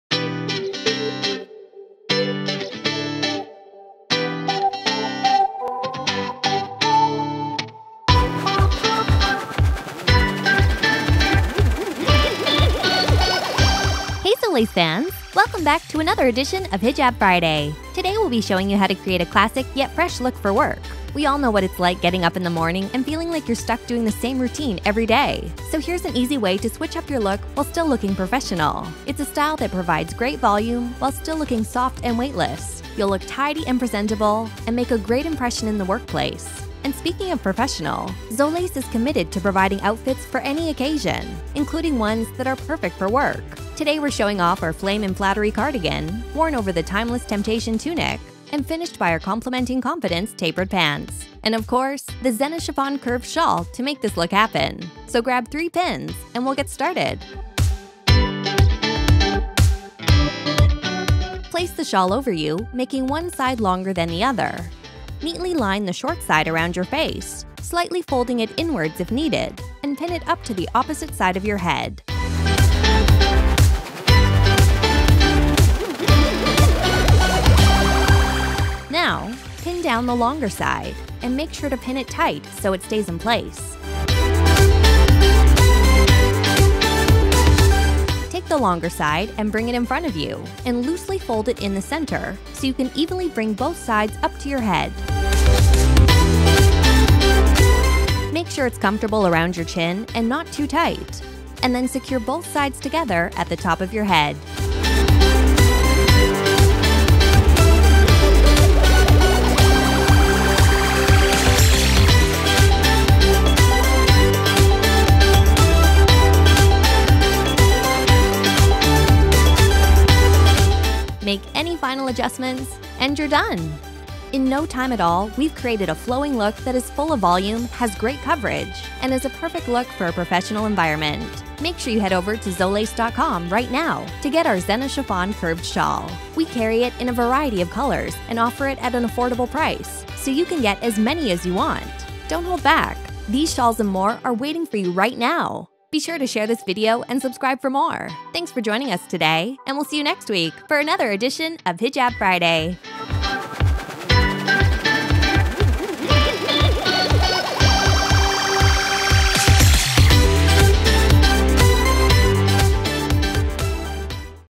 Hijab Shawl Tutorial 2017 - The Best Professional Shawl Style You Can Wear To Work
Video Transcription

Today we’ll be showing you how to create a classic yet fresh look for work. We all know what it’s like getting up in the morning and feeling like you’re stuck doing the same routine every day. So here’s an easy way to switch up your look while still looking professional. It’s a style that provides great volume while still looking soft and weightless. You’ll look tidy and presentable and make a great impression in the workplace.

And speaking of professional, Zolace is committed to providing outfits for any occasion, including ones that are perfect for work. To complete this look, we’ve paired it with the Zehna Chiffon Curved Shawl code Forest with Flame and Flattery Cardigan worn over the Timeless Temptation Tunic , and finished by our Complementing Confidence Tapered Pants .

So just grab 3 pins and we will get started!

Step 1 :Place the shawl over you

Start by placing the shawl over you making one side longer than the other.

Step 2 :Pin the short side of the shawl

Neatly line the short side around your face, slightly folding it inwards if needed and pin it up to the opposite side of your head.

Step 3 :Pin down the longer side to your face

Now, pin down the longer side and make sure to pin it tight so it stays in place.

Step 4: Loosely fold it in the centre

Take the longer side and bring it in front to you, and loosely fold it in the centre so you can evenly bring both sides up to your head.

Step 5: Secure both sides together

Make sure it’s comfortable around your chin and not too tight, and then secure both sides together at the top of your head.

Make final adjustments and you’re done! In no time at all we’ve created a flowing look that is full of volume, has great coverage, and is a perfect look for a professional environment.

Hijab Friday Release Schedule:
We release our weekly Hijab Friday on Friday GMT+8 12PM.